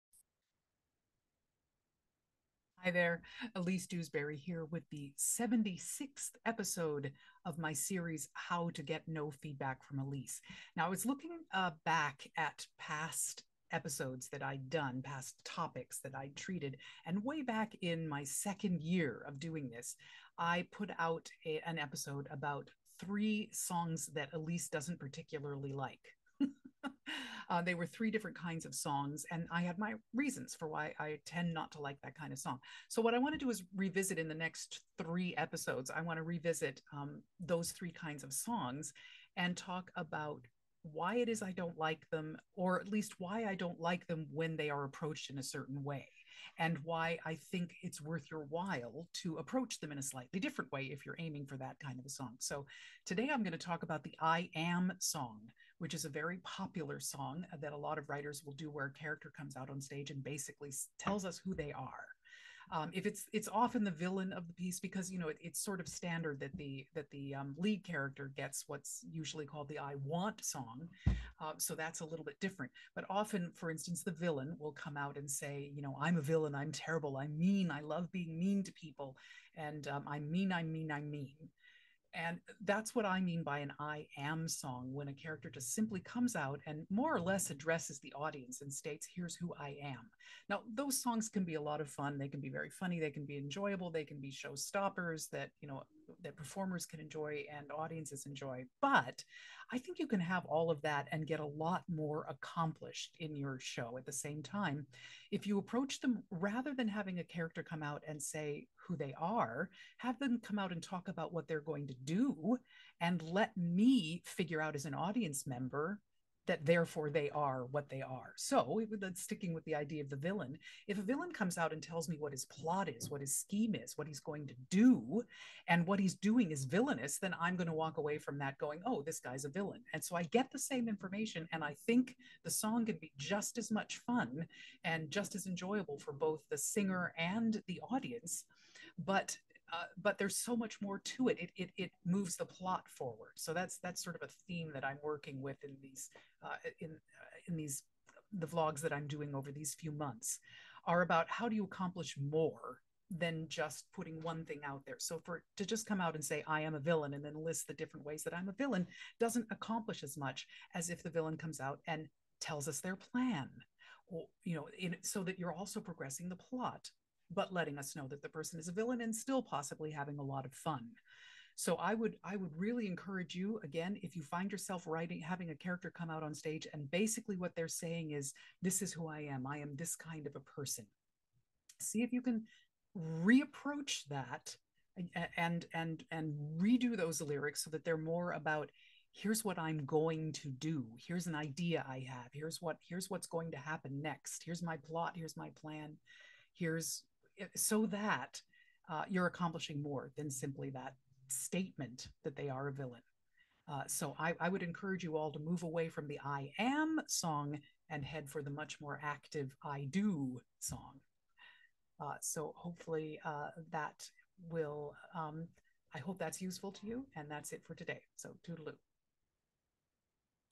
How to Get NO Feedback from Elise: Vlog 76 - ACTIVATING THE "I AM" SONG
The 76th post in the video blog from NMI Artistic Director Elise Dewsberry - How to Get NO Feedback from Elise.  Activating the "I Am" song and why you should do it.

We create new musicals.